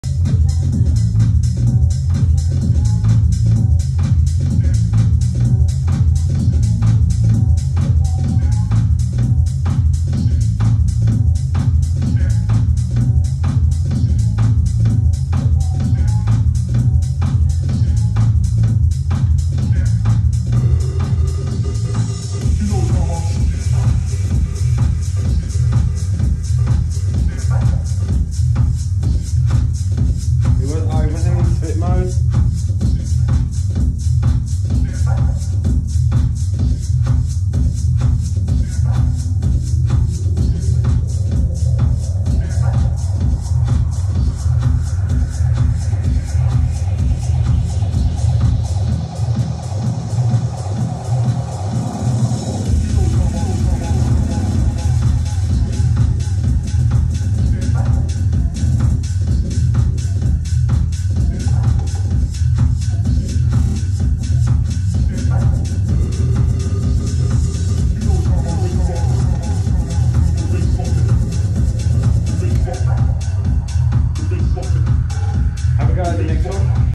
Dj Jasmine Rose 16th Oct 2022
DJ lesson with Marc Spence
If at first you don't succeed try try again
Trying to getting timings right with the odd slip loop